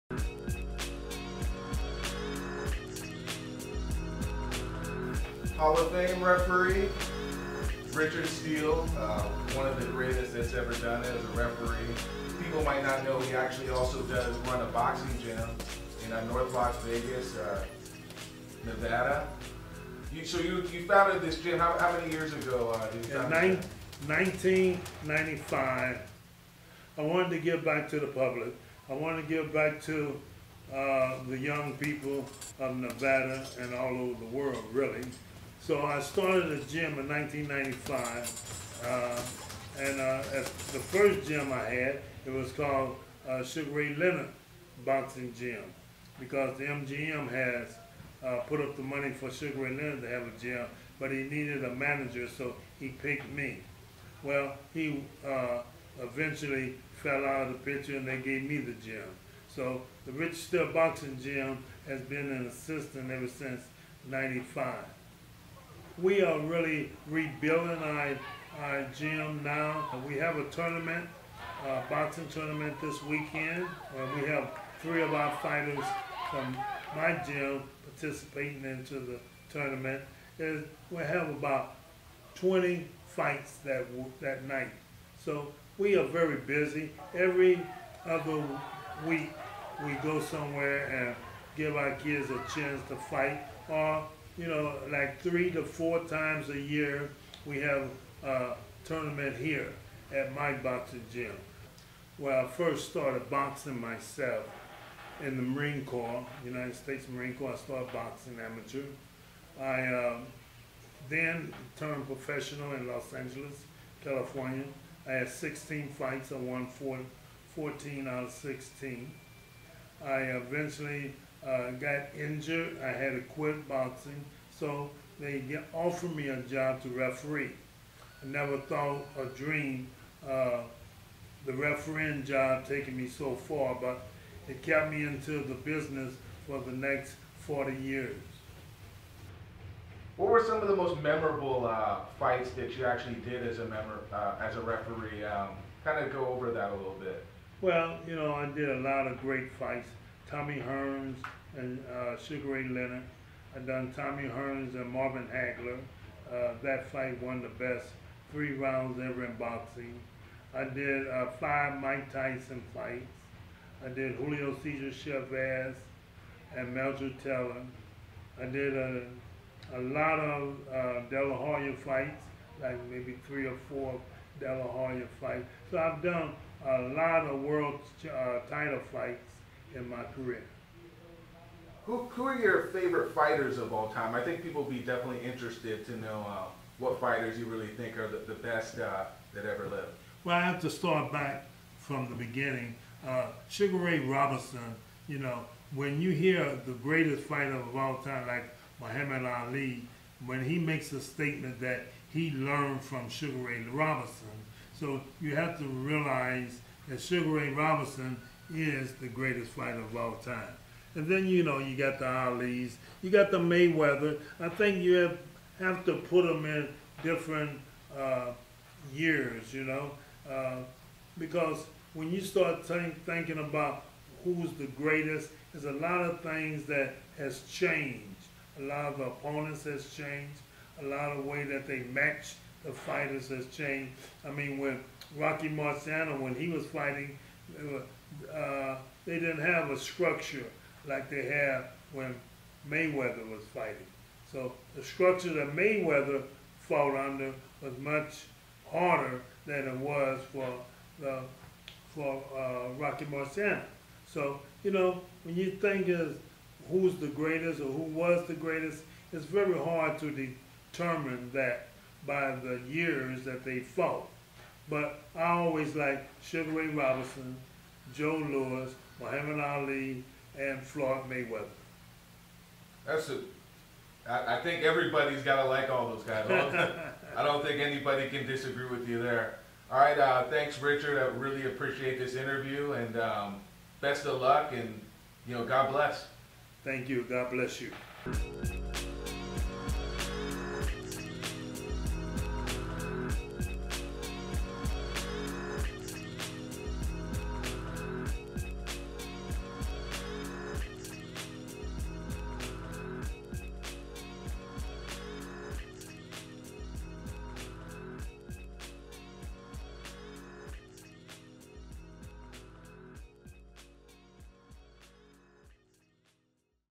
Richard Steele Short Documentary/Interview
Kevin Perry of Kayonation.com interviews Hall of Fame Referee Richard Steele at the Richard Steele Boxing Club in Las Vegas, NV. He currently works with youths in the Las vegas area and operates the gym and the Richard Steele Foundation.